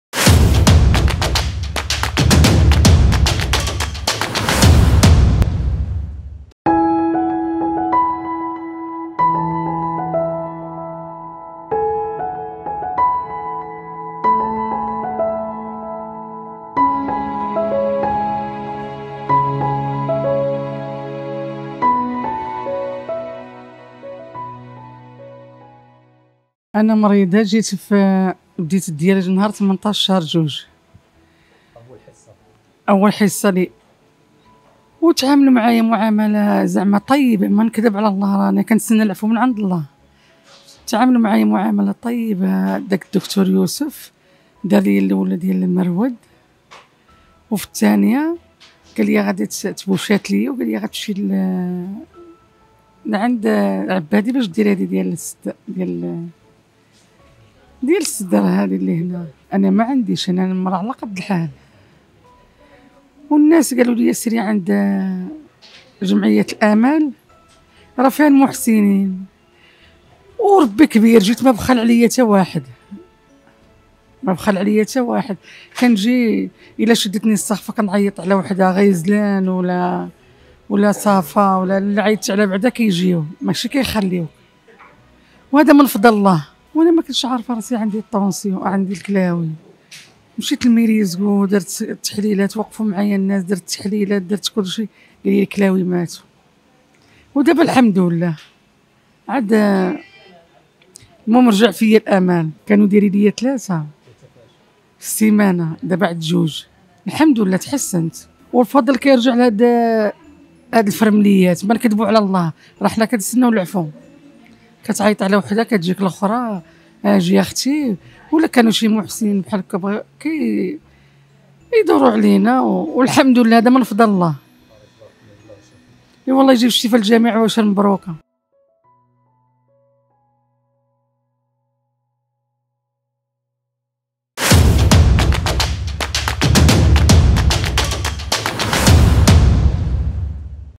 مؤسسة أمل تزرع الأمل في مرضى القصور الكلوي
لا يختلف اثنان حول الخدمات التي تقدمها مؤسسة أمل لعلاج مرضى القصور الكلوي بالدار البيضاء وغيرها من المراكز..
"أنفاس بريس"، زارت المركز الرئيسي للمؤسسة بتراب عمالة مقاطعة ابن امسيك، حيث كان العشرات من المرضى يتلقون علاجاتهم تحت عناية طبية وتمريضية..

# أخبار_عاجلة